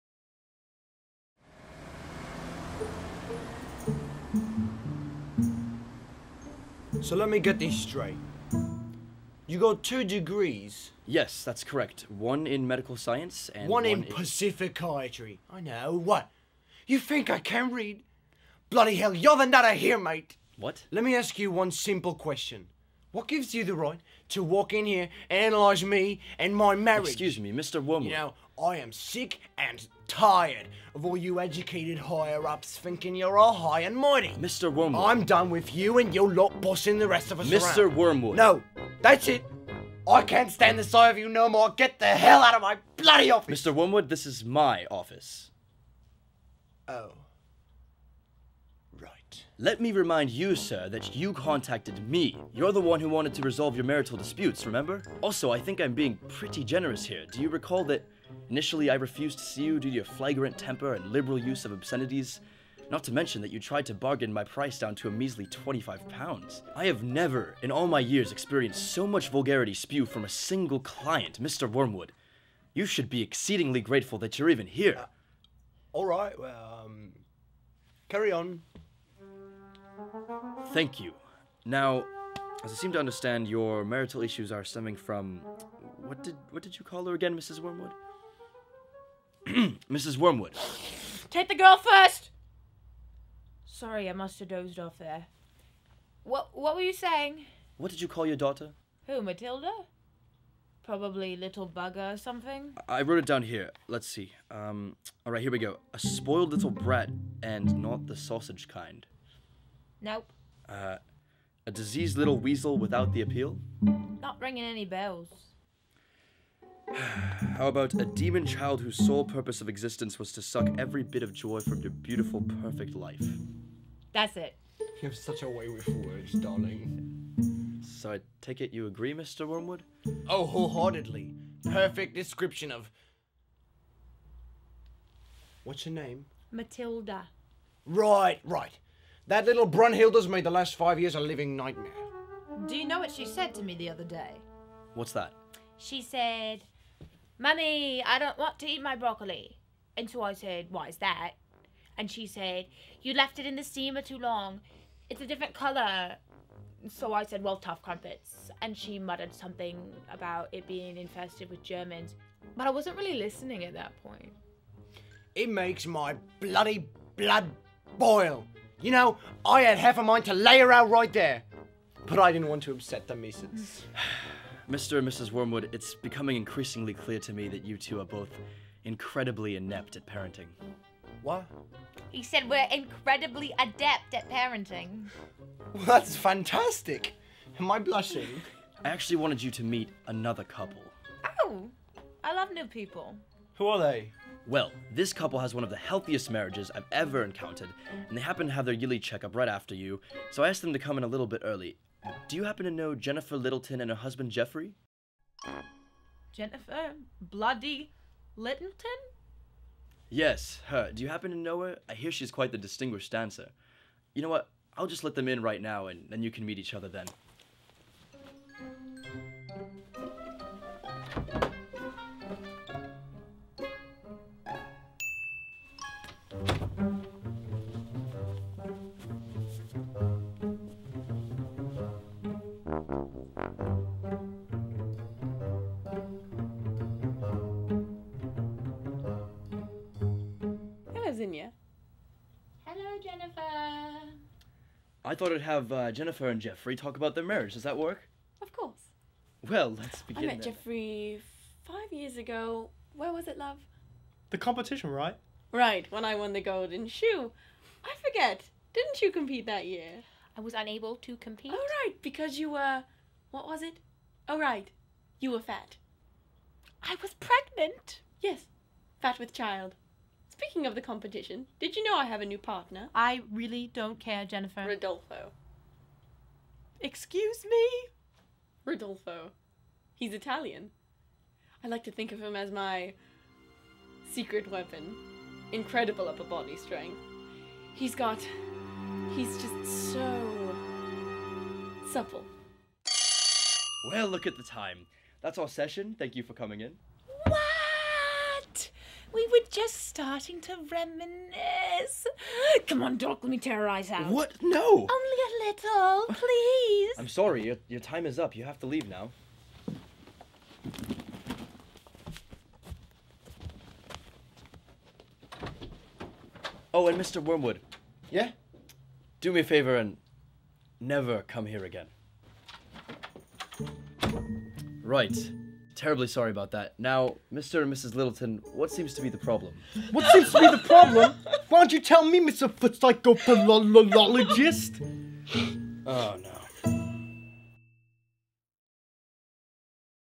Marriage Counseling With The Wormwoods, Music of the Night 2015 Video Short
Marriage Counseling With The Wormwoods, created for Santa Barbara High School Theatre's "Music of the Night" a student directed Broadway Revue. Directed by Alessio Morello, filmed by Beau Lettieri. Written by Nick Blondell and Aaron Linker.